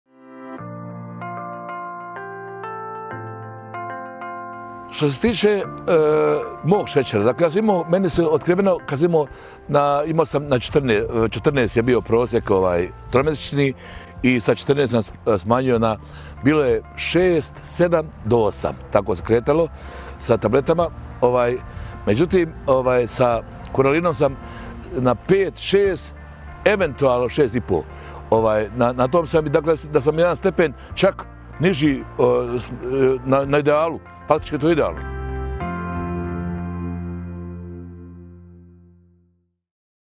CuraLin Halid Bešlić
Oduševljen sam! Prednost CuraLin-a je i to što je prirodan, dakle od prirodnih sastojaka, a vrlo je efikasan. Koristim ga već 4 mjeseca i moj šećer je u normalnim vrijednostima.